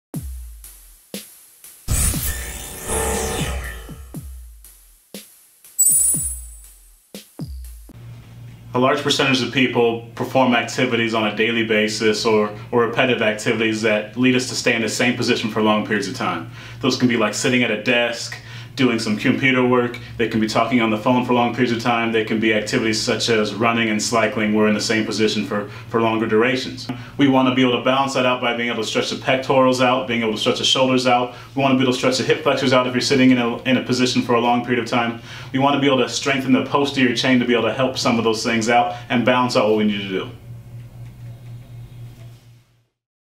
Limit the Negative Effects of Repetitive and Chronic Activities
Avoid the negative effects of sitting, standing, slumping, or performing repetitive activities by balancing them out by stretching and strengthening the opposite motions to keep better health and limit potential for injury.